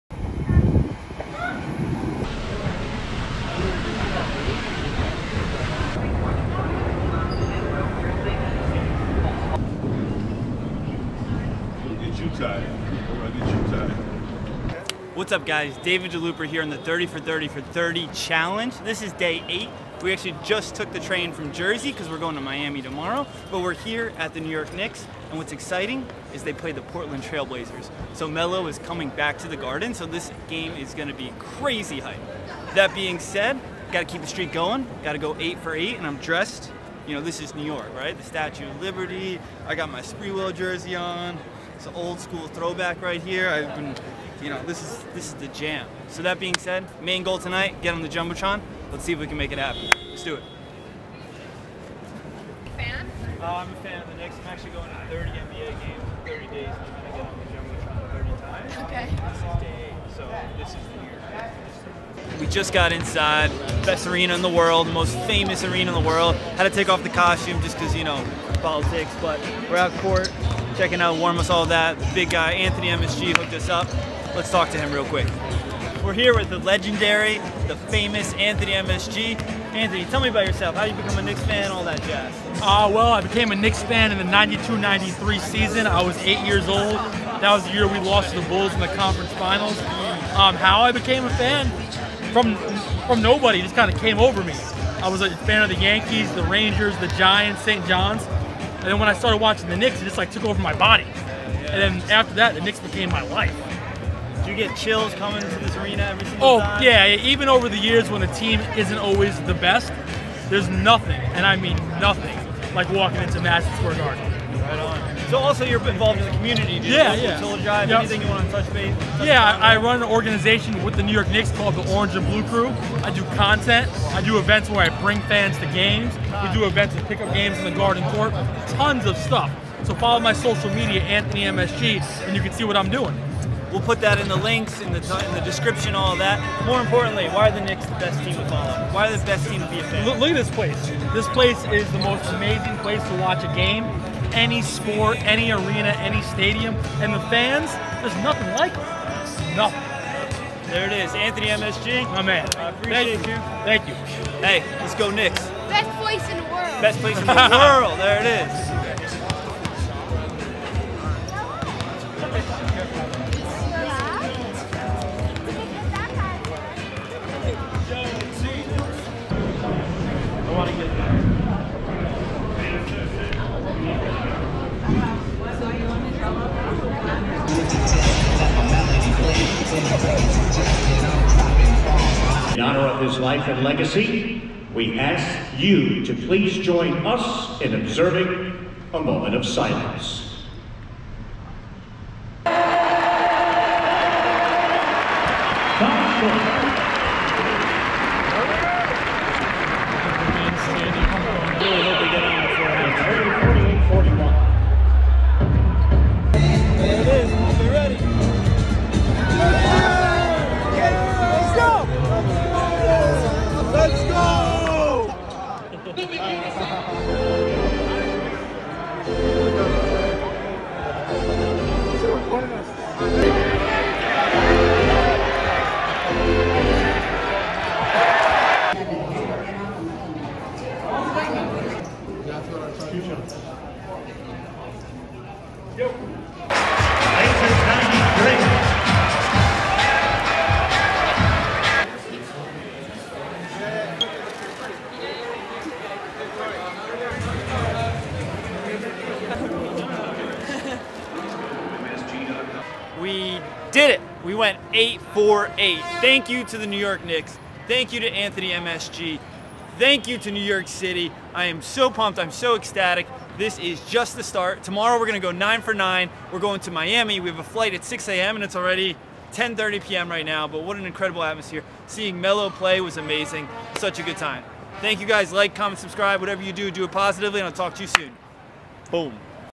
I Made It On The Jumbotron At The New York Knicks| 30for30for30
I'm officially 8 for 8 in my attempts of getting on the Jumbotron, so far. This time it was Madison Square Garden for the Knicks vs Trail Blazers. It was Carmelo Anthony's second time back at the Garden and the fans showed their respect. Since I was in New York City, I just had to dress as the Statue of Liberty... with a Spreewell jersey on. Let me know what you think of my costume in the comments below!